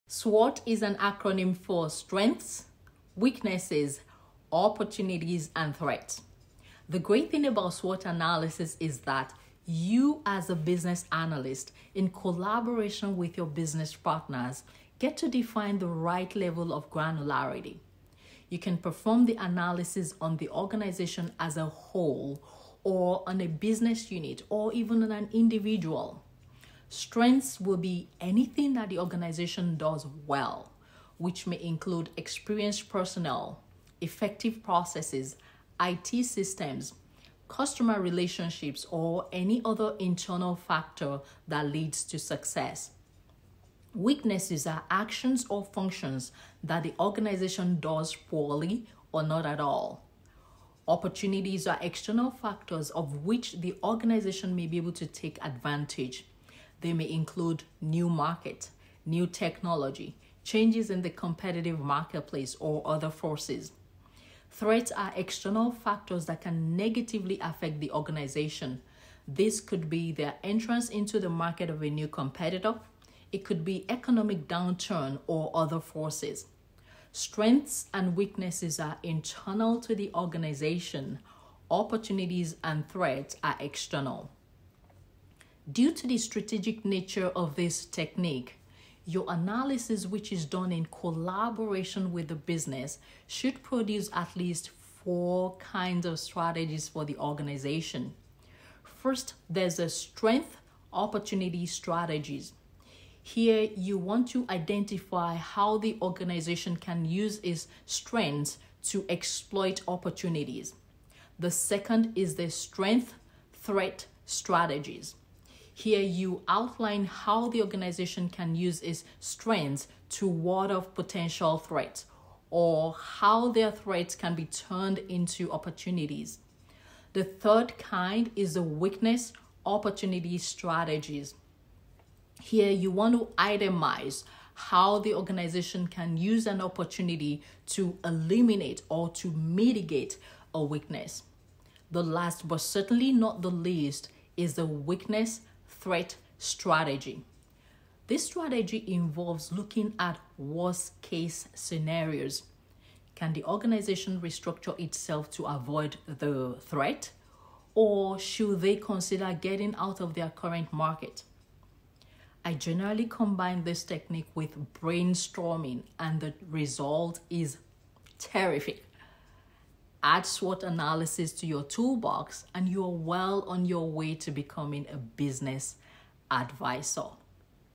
SWOT Analysis
SWOT is an acronym for Strengths, Weaknesses, Opportunities, and Threats.
You can perform the analysis on the organization as a whole, or on a business unit or even on an individual.